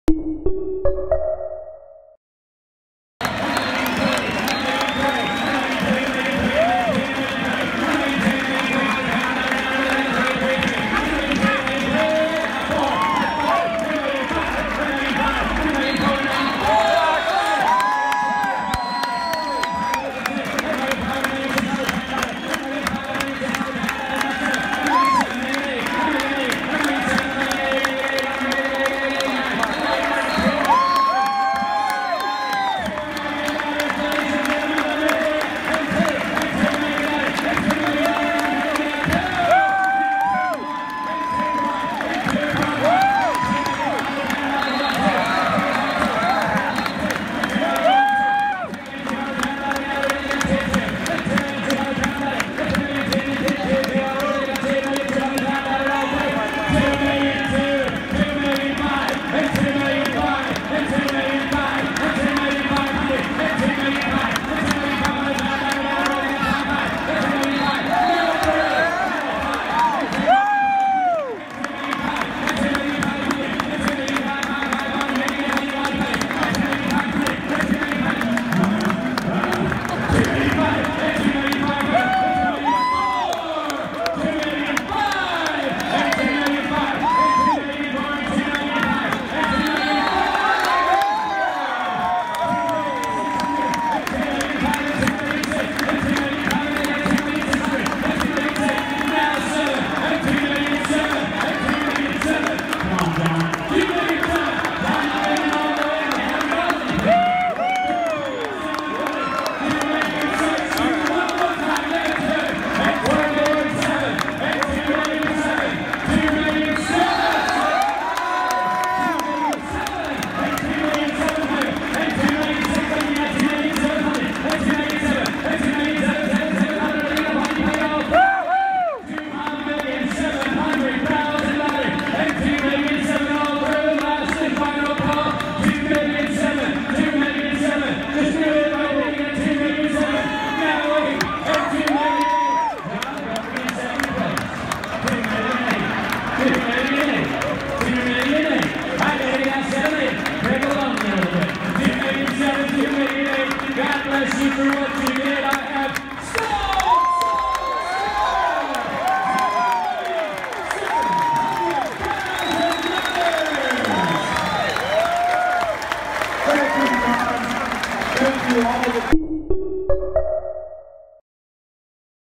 Watch The Hammer Fall At $2.7 Million For The Final C7 Corvette
The final seventh-generation Corvette ever produced went up for auction at the Barrett-Jackson Northeast event and sold for $2.7 million, making it the most expensive car ever sold at auction for charity. This video puts you on the stage as the bidding went from $1 million to when the gavel fell at $2.7 million.